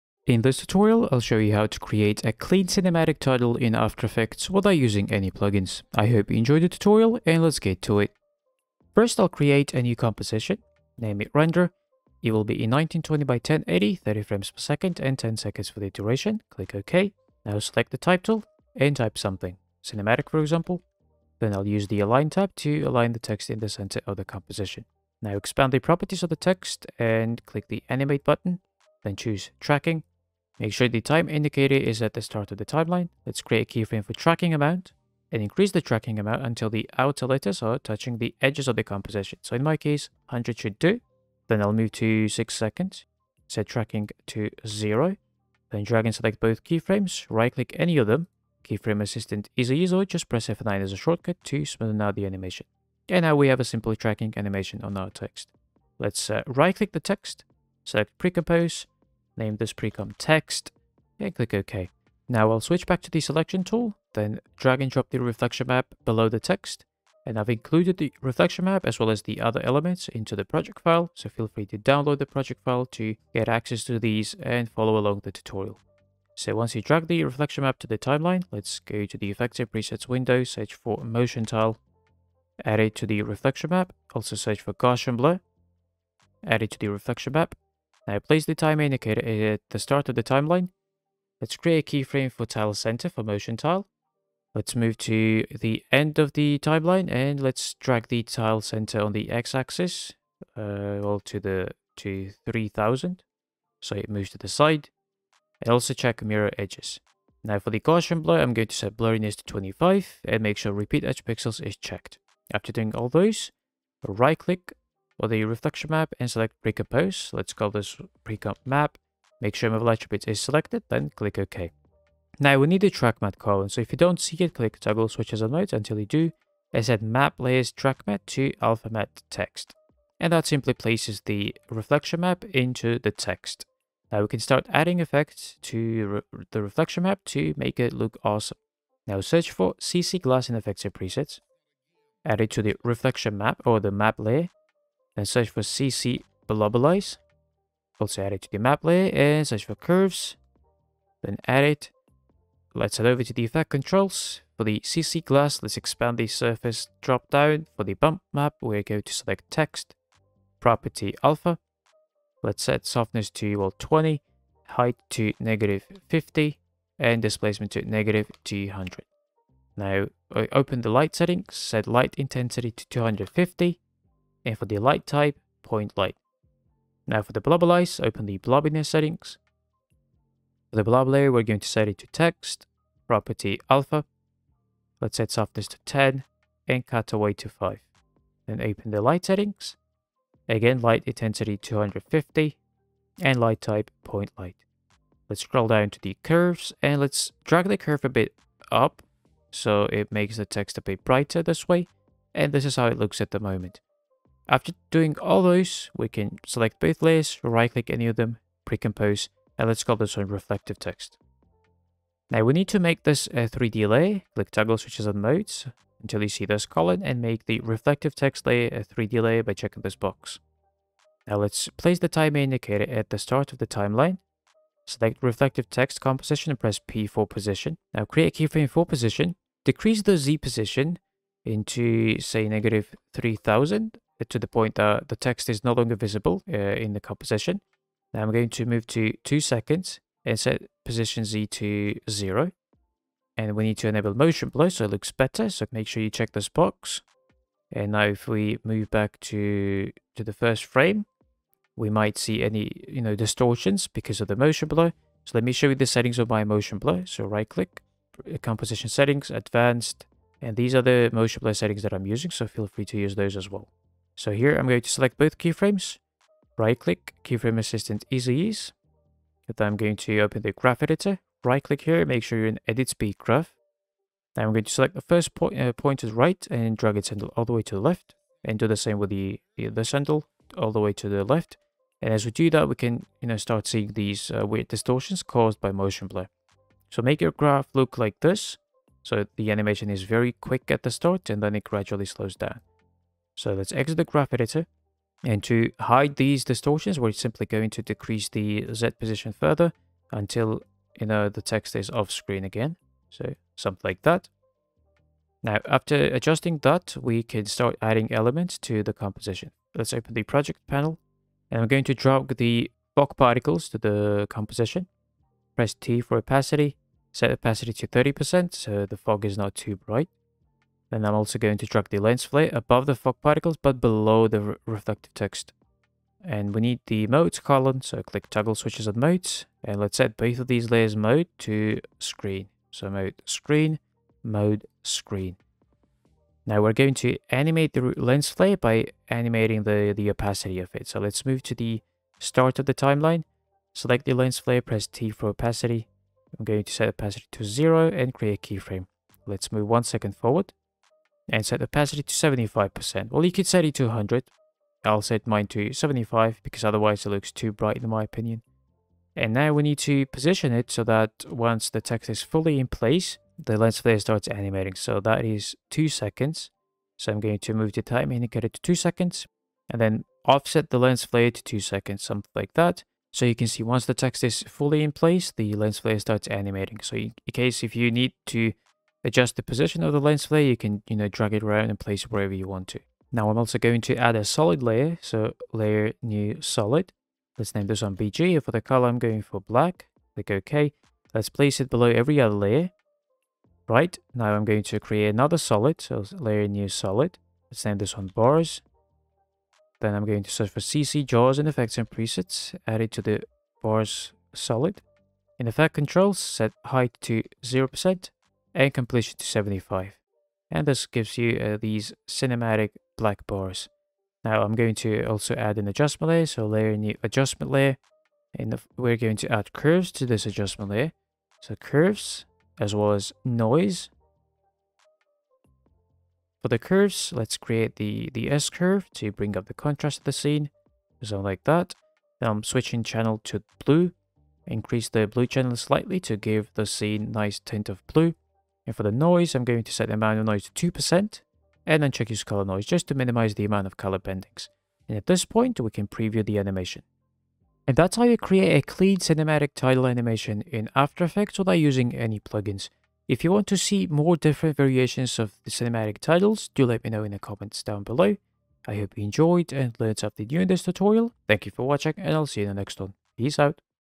Cinematic Trailer Title Animation Tutorial in After Effects | No Plugins
In this tutorial I show you how to create cinematic trailer title animation in adobe after effects without using any plugins. Method shown in this video is super simple and quick, I included all assets used into the project file, download it to follow along.